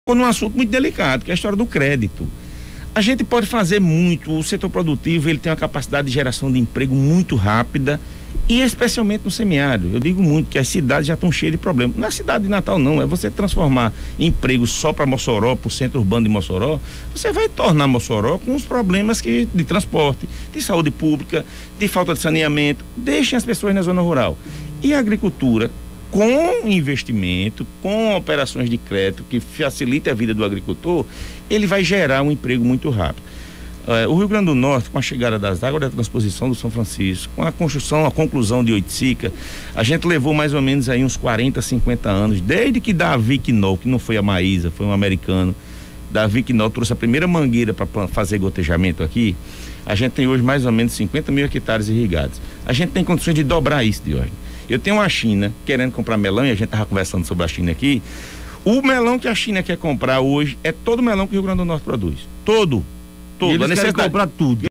Crédito para o agricultor potiguar | Guilherme Saldanha (SAPE/RN) - 21.02.2022
O emprego do crédito pelos agricultores para a manutenção de suas atividades, veja o que o secretário de Agricultura, Pecuária e Pesca do RN diz sobre o assunto.

As notícias mais importantes do dia estão aqui!